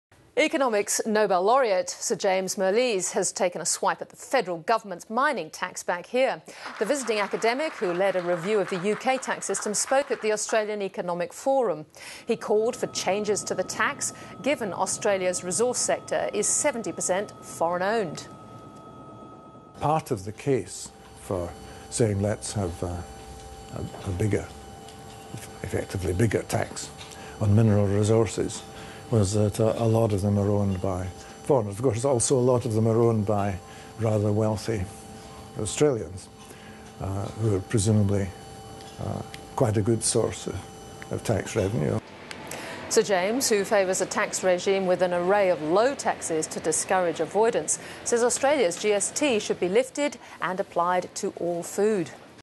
Sir Mirrlees calls for GST on food
Scottish Nobel Laureat Sir James Mirrlees has called for changes to Australia's mining tax and for an increase to the GST.